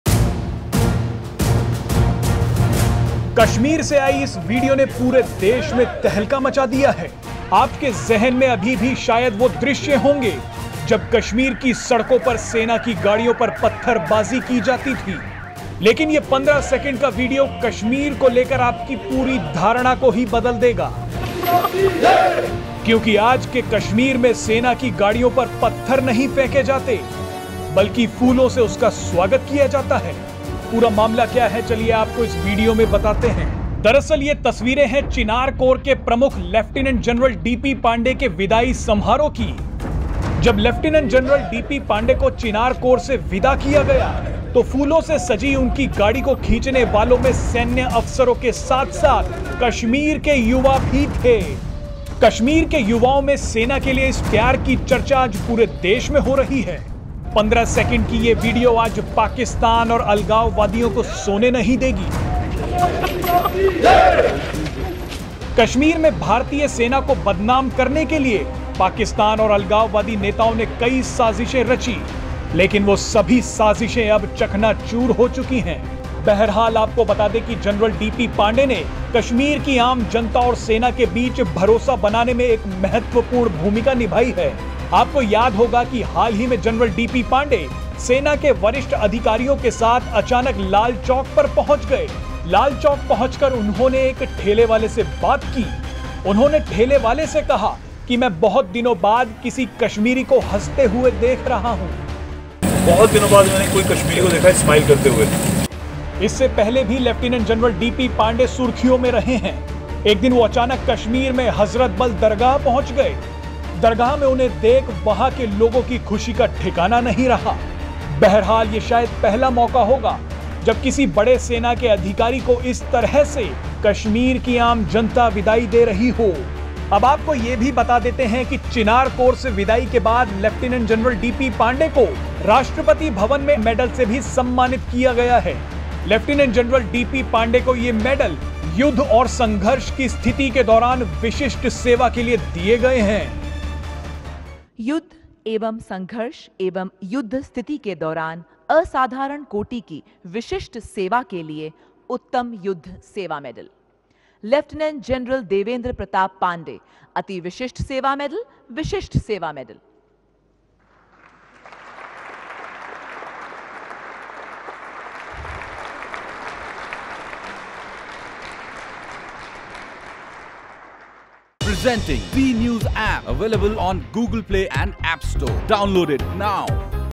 कश्मीर से आया 15 सेकंड का वीडियो, दुनिया में तहलका मचाने को काफी |DP Pandey Chinar Corps Indian Army
Lt Gen Pandey farewell video viral on internet. Lt Gen Pandey complimented all ranks of Chinar Corps for their dedication and hard work. He appreciated JKP, CAPFs, civil administration and community members for their relentless support in the joint effort towards peace and prosperity in J-K.

लेफ्टिनेंट जनरल डी पी पांडे अपने एक साल का कार्यकाल पूरा करने के बाद अब श्रीनगर से सेना के महू (मध्य प्रदेश) स्थित वॉर कॉलेज के कमांडेंट के तौर पर जा रहे हैं. उनकी जगह पर लेफ्टिनेंट जनरल अमरदीप सिंह ओजला अब चिनार कोर की कमान संभालेंगे. सोमवार को लेफ्टिनेंट जनरल पांडे ने चिनार कोर का कार्यभार लेफ्टिनेंट जनरल ओजला को सौंप दिया. लेफ्टिनेंट जनरल डी पी पांडे की विदाई का एक वीडियो अब सोशल मीडिया पर काफी वायतल हो रहा है. क्या है ये पूरा मामला जानिए इस वीडियो में...